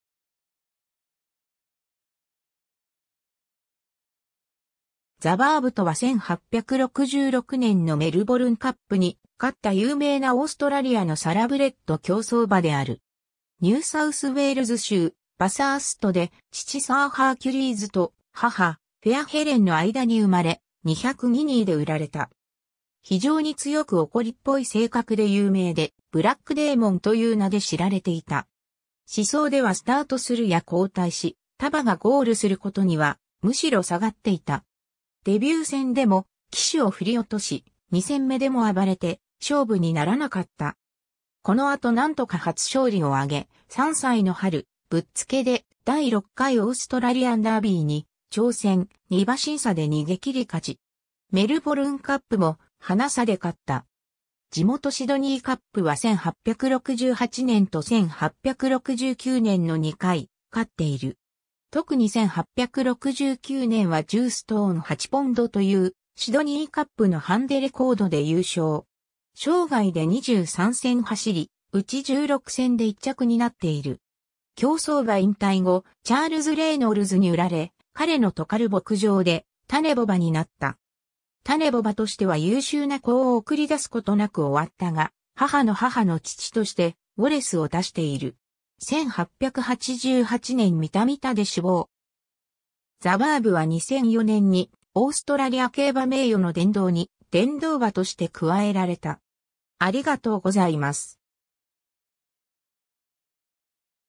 ザバーブ
ザバーブ（The Barb、1863年 - 1888年）とは1866年のメルボルンカップに勝った有名なオーストラリア（当時はイギリス領ニューサウスウェールズ州）のサラブレッド競走馬である。
 ニューサウスウェールズ州・バサーストで、父サーハーキュリーズと母フェアヘレンの間に生まれ、200ギニーで売られた。
非常に強く怒りっぽい性格で有名で「ブラックデーモン」という名で知られていた。
試走ではスタートするや後退し、他馬がゴールすることにはむしろ下がっていた。
デビュー戦でも騎手を振り落とし、2戦目でも暴れて勝負にならなかった。
このあとなんとか初勝利を上げ、3歳の春（つまり9月）、ぶっつけで第6回オーストラリアンダービーに挑戦、2馬身差で逃げ切り勝ち。
メルボルンカップもハナ差で勝った。
地元シドニーカップは1868年と1869年の2回勝っている。
とくに1869年は10ストーン8ポンド（約67kg）というシドニーカップのハンデレコードで優勝。
生涯で23戦走り、うち16戦で1着になっている（22-24戦、15-17勝と推定されているが、記録がはっきりとは残っていない）。
 競走馬引退後、チャールズ・レイノルズに売られ、彼のトカル牧場で種牡馬になった。
種牡馬としては優秀な子を送り出すことなく終わったが、母の母の父としてウォレス（カーバインのオーストラリアでの最良馬。ヴィクトリアダービー、ヴィクトリアセントレジャー、シドニーカップ、豪リーディングサイアー5回）を出している。
1888年ミタミタで死亡。
ザバーブは2004年にオーストラリア競馬名誉の殿堂に殿堂馬として加えられた。